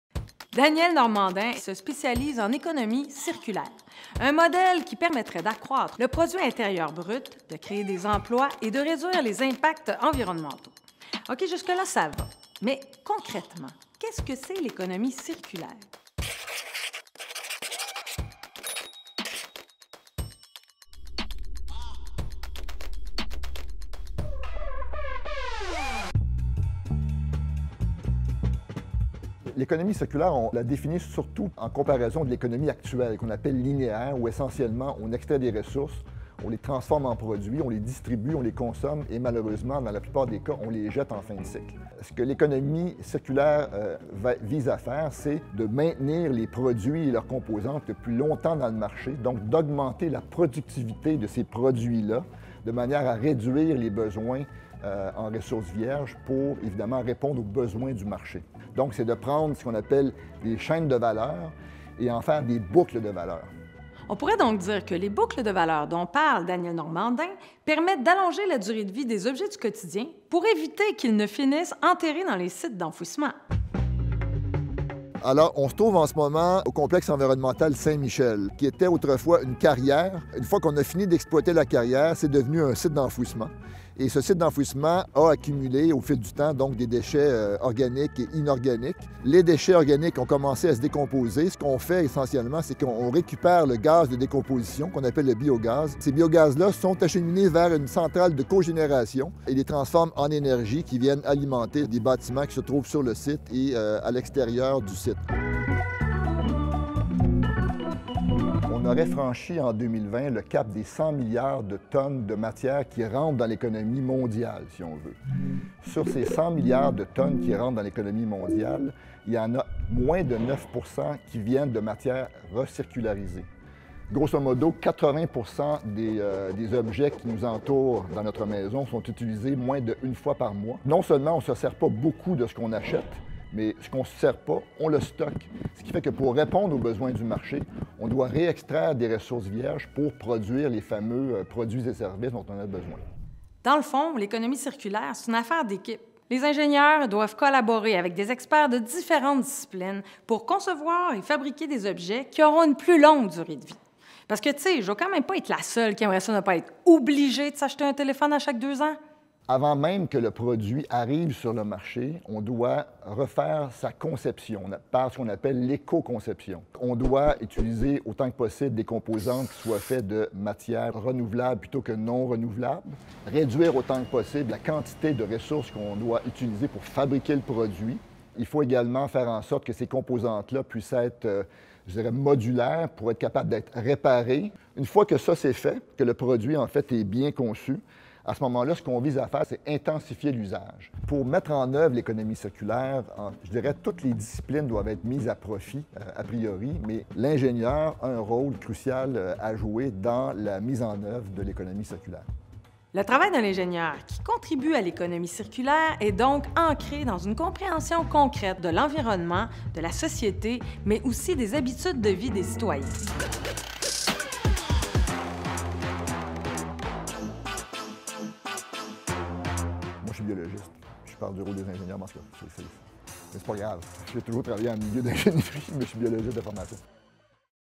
L'économie circulaire - Coups de génie | ÉTS Montréal
Daniel Normandin, directeur du Ceriec, explique les fondements de l'économie circulaire et comment cela prendrait forme en réduisant nos demandes en ressources vierges, et en repensant les produits pour en prolonger la vie utile et en y intégrant des matières post-consommation.